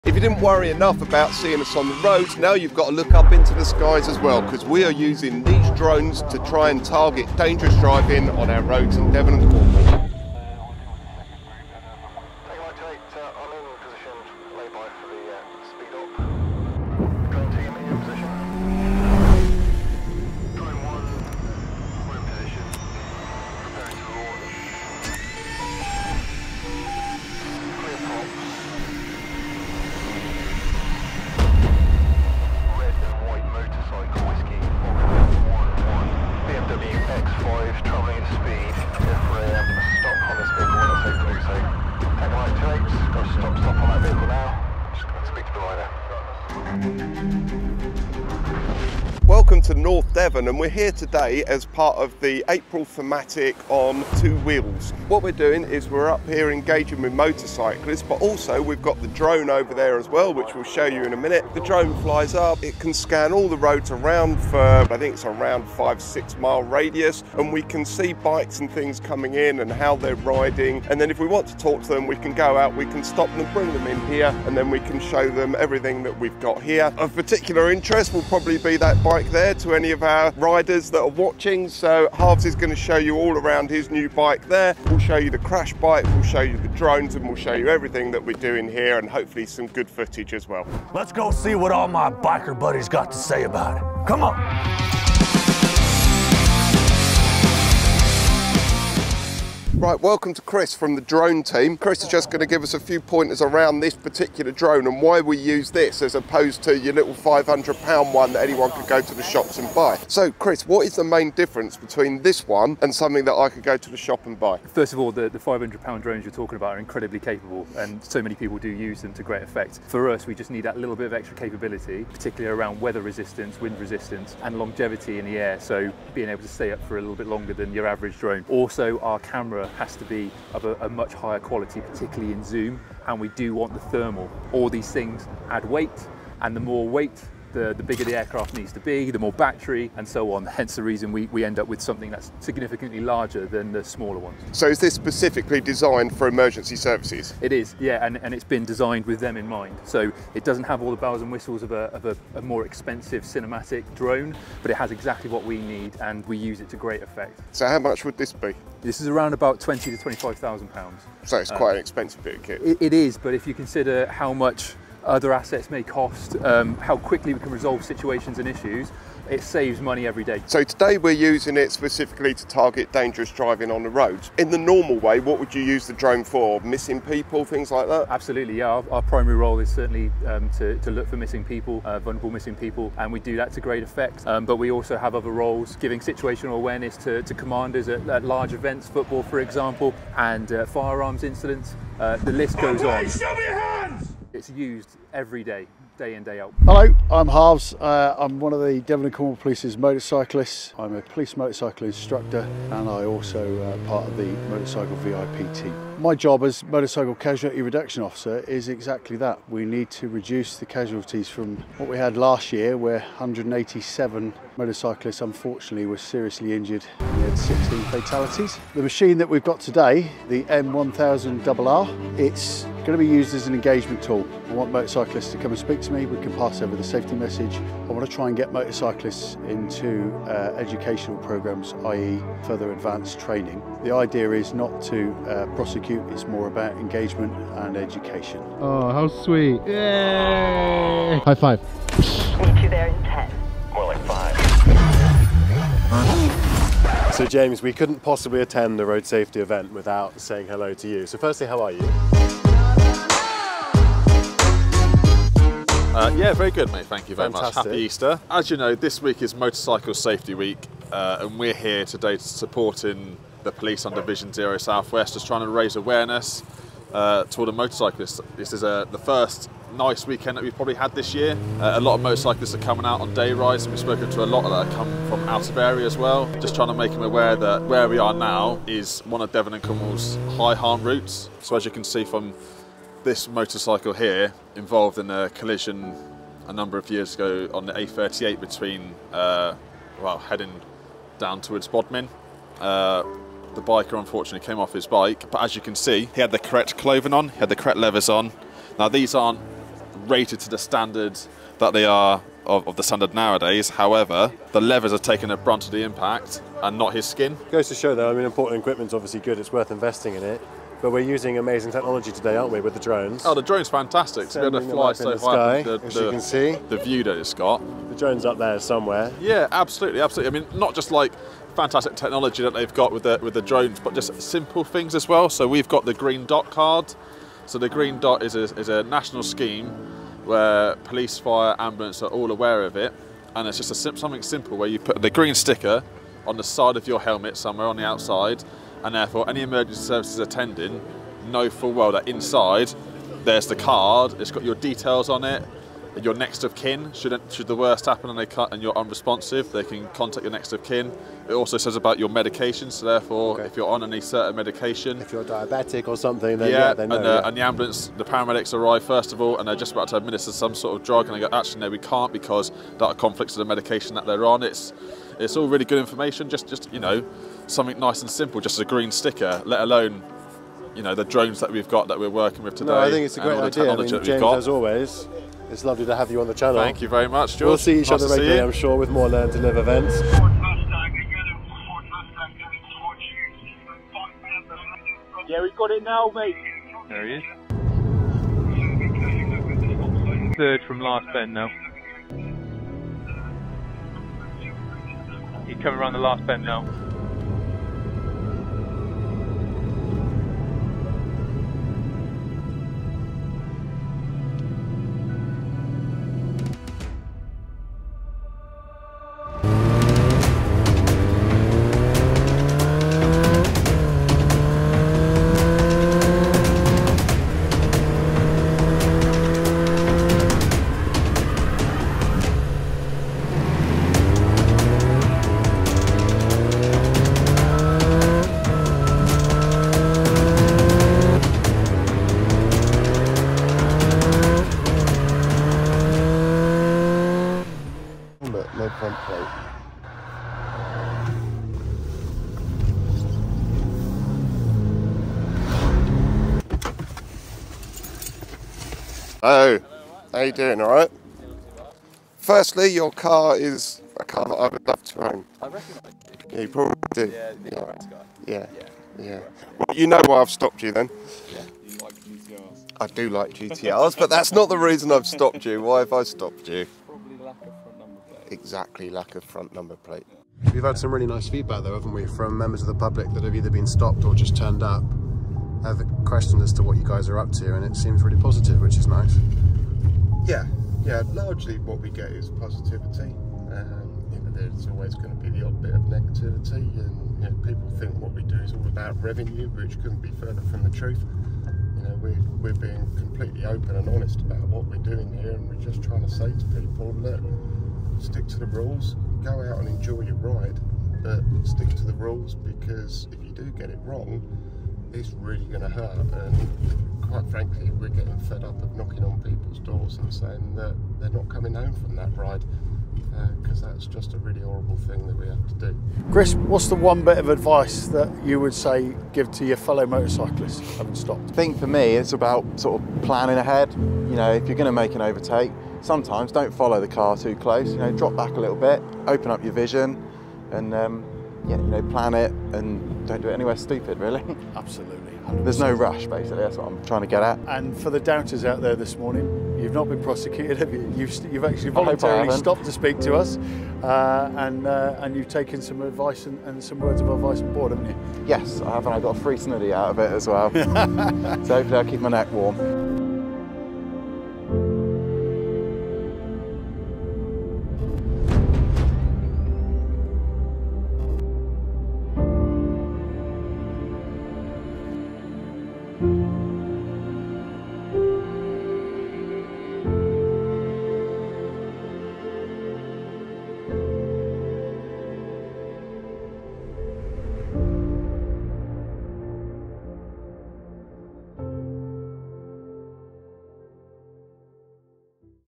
POLICE DRONES!! - Dangerous drivers WATCH OUT!!!
In today’s video, we explore the innovative ways that the D&C police are using DJI drones, costing more than £25,000, to combat dangerous driving with cars & motorbikes, and how they are also using them to find missing people using thermal imaging.

The DJI M300 RTK is a next-level commercial drone platform that offers enhanced flight performance and unrivalled safety features. With a 55-minute maximum flight time, advanced AI capabilities, six directional sensors, and positioning, and the ability to carry three payloads at once - including hybrid camera solutions designed especially for the platform - this IP45-rated drone sets a new standard, combining intelligence with reliability.

The DJI M300 RTK is a versatile drone and has become an important platform for public safety, search and rescue, industrial inspection, aerial surveying, and utilities/oil and gas, among other applications, equipped with the latest technology, including high-definition cameras, thermal imaging, and GPS tracking, making them an invaluable asset to the police force. Join us as we delve into the specifics of how these drones are used, how they have impacted the police force's operations, and what the future of law enforcement looks like with this cutting-edge technology.

If you're interested in the latest advancements in drone technology and how they are being utilized for public safety, this video is a must-watch.

The BMW M1000RR will for sure be featured in future videos, so keep your eyes peeled.

Big thanks to everyone involved in today's video, including all the kind members of the public that came to say hello and ask such nice questions.

Vision Zero Southwest and Xpressmedia also worked very hard behind the scenes for some really valuable parts in the video, so thank you.